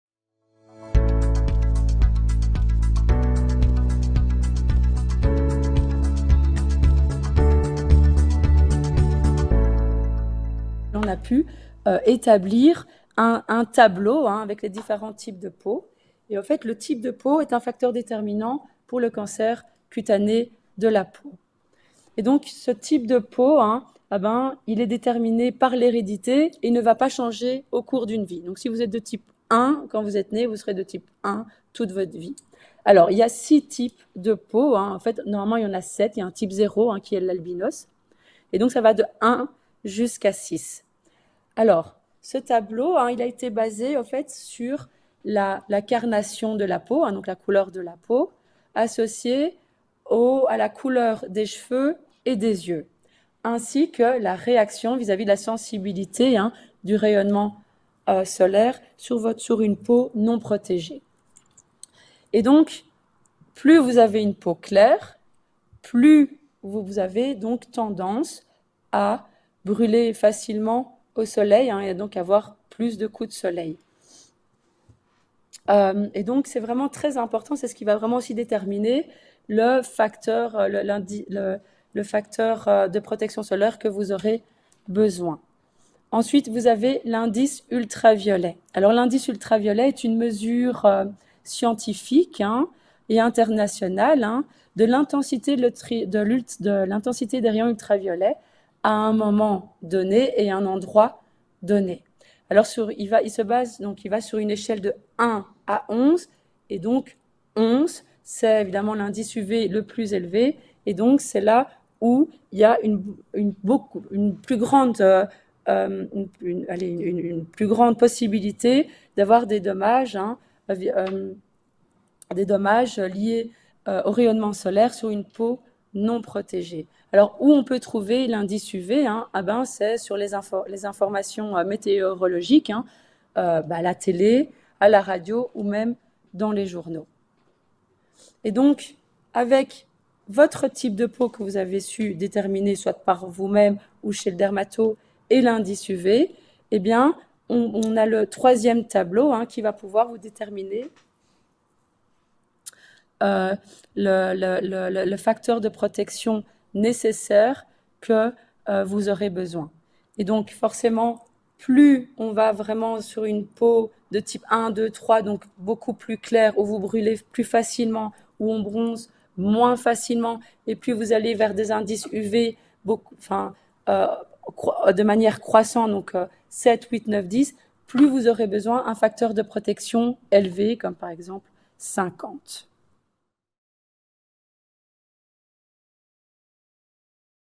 Let's Talk « Peau & Soleil » : Les rayons uv
Notre dernière séance Let’s Talk s’intéressait aux effets du soleil sur la peau. Quelles sont les différents types d’UV ? Quelle méthode utiliser pour identifier un grain de beauté à risque ? Quelles sont les différents types de peau ? Comment bien les protéger ? Qu’est-ce que la phototoxicité ? Pendant une heure, Anne et Noé, nos pharmaciennes expertes ont répondu à ces questions mais aussi celles de nos participant(e)s.

Quelles sont les différents rayonnements qui émanent du soleil ? Quel est votre type de peau. Dans cette vidéo, nos pharmaciennes vous aident à mieux comprendre d’où provient le bronzage et pourquoi certaines personnes brûlent plus vite que d’autres.